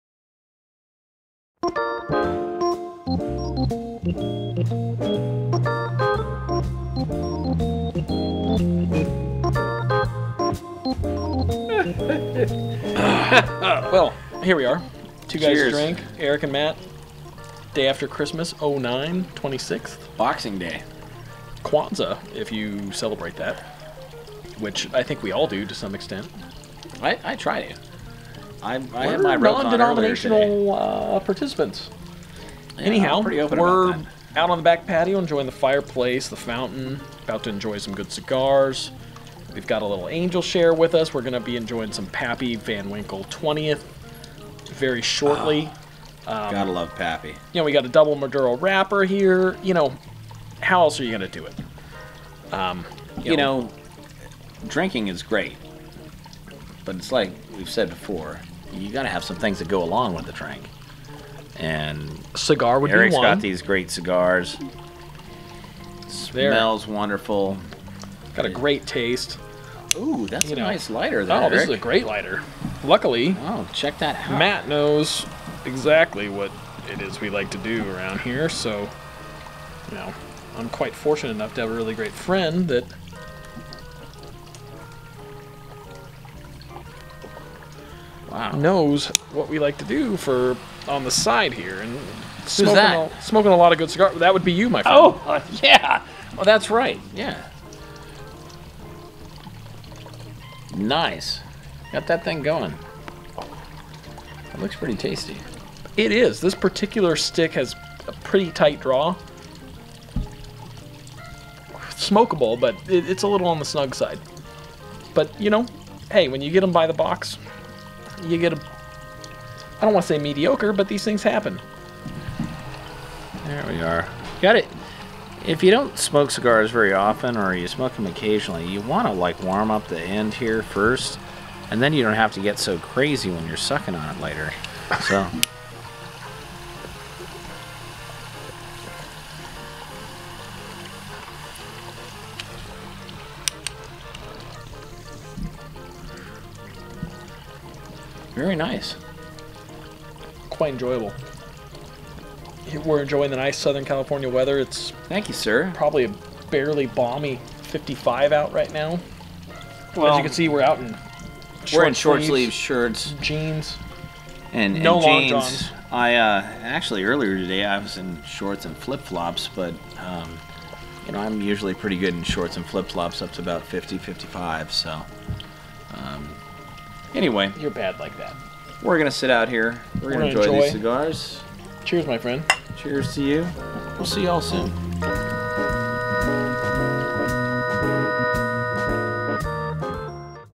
Cigar Time!
Eric and Matt enjoy a Maduro Wrapper while sitting by the fire and drinking some 2006 Lost Abbey Angel's Share.